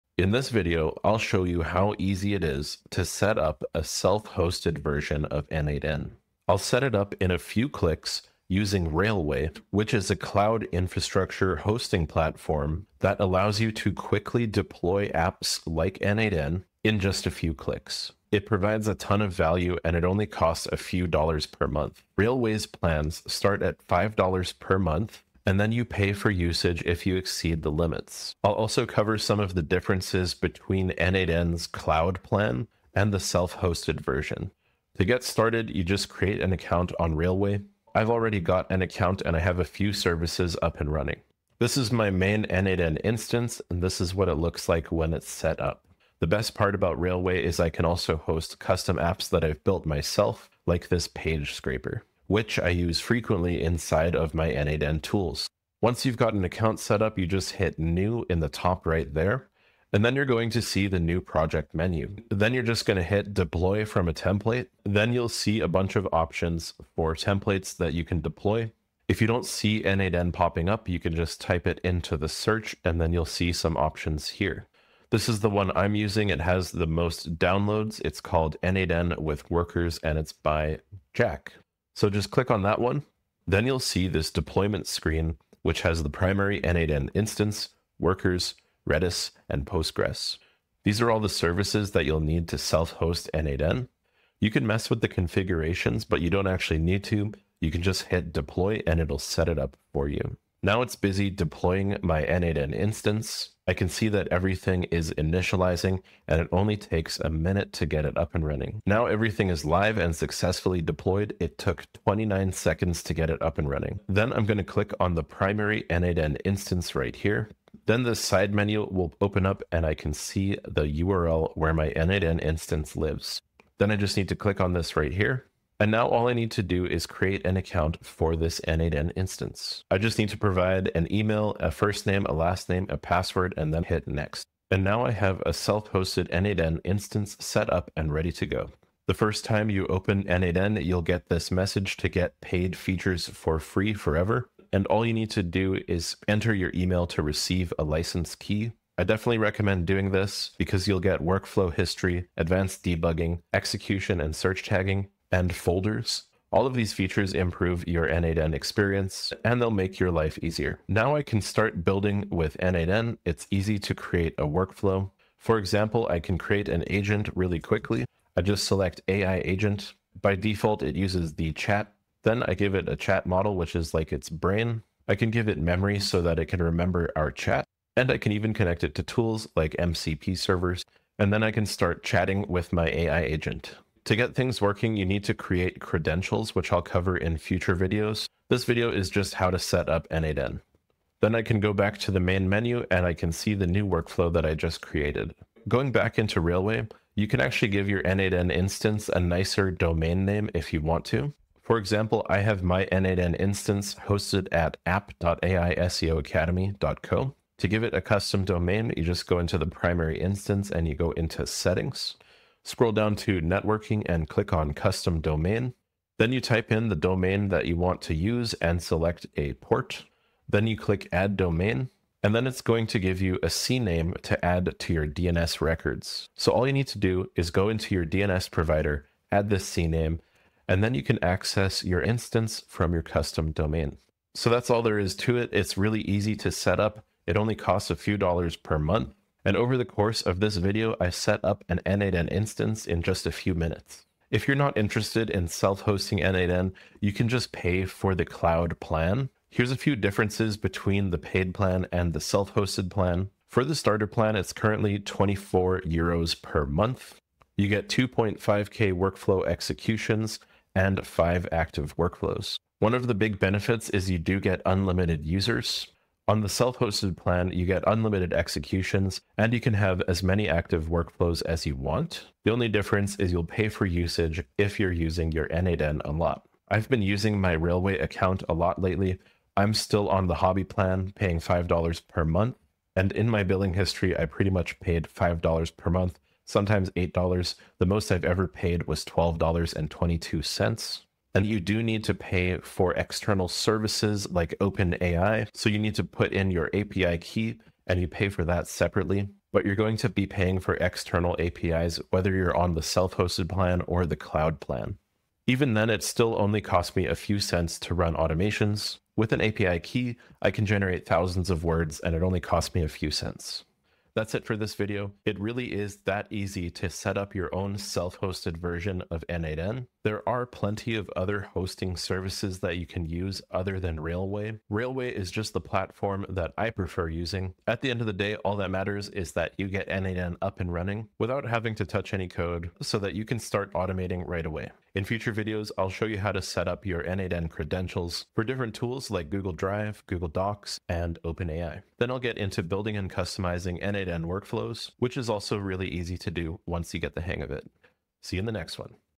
How To Set Up Self-Hosted N8N In 5 Minutes With Railway
Learn how to self-host N8N on Railway in just a few minutes and unlock unlimited workflow automations at a fraction of the cloud cost. I’ll walk you through creating a Railway account, deploying N8N using a template, configuring your instance, and even setting up a custom domain.

Many people stick with the official cloud plan and run into limits, but self-hosting with Railway gives you full control and scalability for as little as $5/month. By the end, you’ll have a fully functional automation platform ready to build AI-powered workflows, connect APIs, and automate your projects without server headaches. Watch until the end to start saving money and running unlimited workflows today.

00:00 Introduction to Self-Hosting N8N
00:34 Setting Up Railway Account
01:02 Deploying N8N on Railway
01:57 Configuring Your N8N Instance
03:15 Custom Domain Setup
04:04 Comparing Cloud and Self-Hosted Plans
05:13 Conclusion and Future Videos